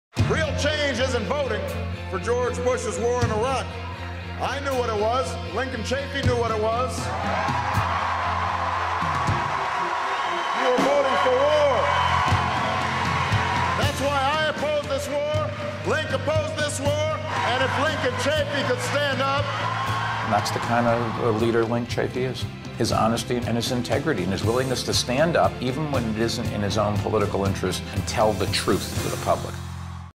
Chafee - Stand Up
Less than 24 hours after Rhode Island Gubernatorial candidate Frank Caprio told President Obama to "take his endorsement and shove it" the Chafee campaign was up on every major broadcast network with "Stand Up" a spot that features then Presidential candidate Barack Obama praising Linc Chafee as an honest leader who is not afraid to "stand up" for the good of the people.  This ad effectively kept voter attention on Caprio's damaging gaffe earning write-ups in CNN's political ticker and the New York Times Caucus Blog.  This spot helped Independent candidate Linc Chafee win the election and become the only Independent Governor in the nation.